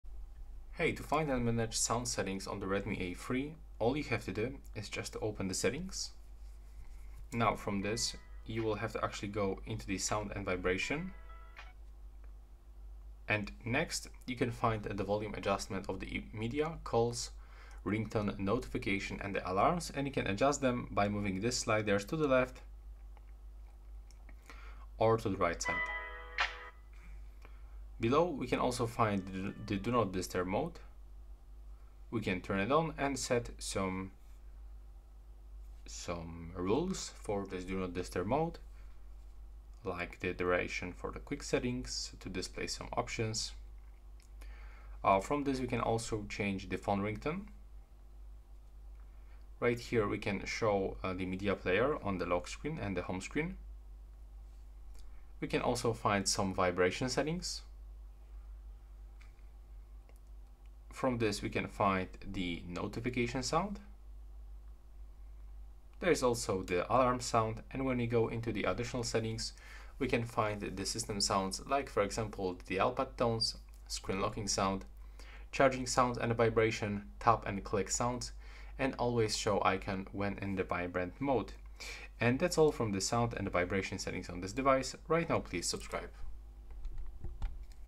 How to Find & Manage Sound Settings on Redmi A3?
Want to personalize your Redmi A3's sound experience for music, movies, calls, and notifications? This video is here to help! We'll guide you through navigating and managing the sound settings on your Redmi A3 with clear visuals and step-by-step instructions.

Redmi A3 Sound Settings
Manage Redmi A3 Sound
Redmi A3 Volume Control
Change Ringtones Redmi A3